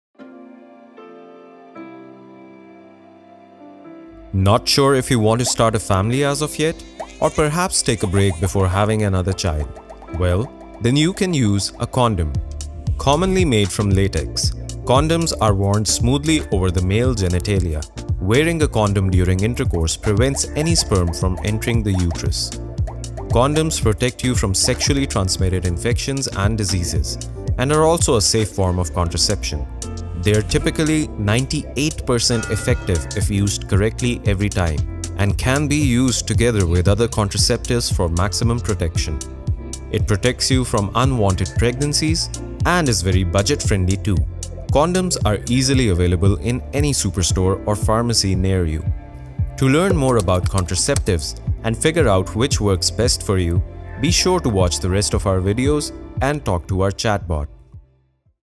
Male Condom
A condom is a thin film sheath that is placed over a man’s erect penis before having sex.